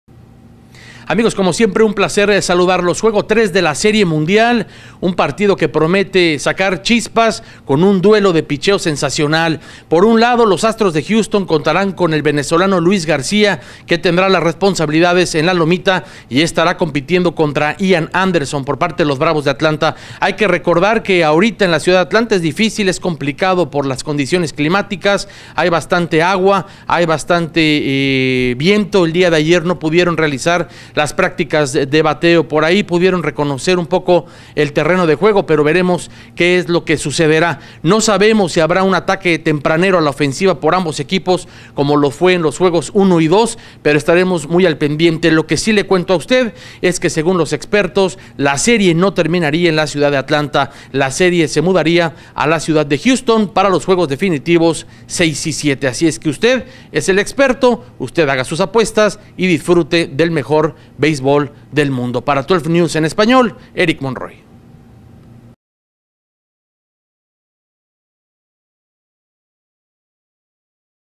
Juego 3 de la Serie Mundial con duelo de lanzadores
El venezolano Luis García tendrá la resposabilidad en la lomita por los Astros de Houston y por los Bravos de Atlanta el lanzador será Ian Anderson.

Ambos tuvieron excelentes salidas en sus últimas apariciones cuando sus equipos lograron victorias para catapultarlos a la Serie Mundial.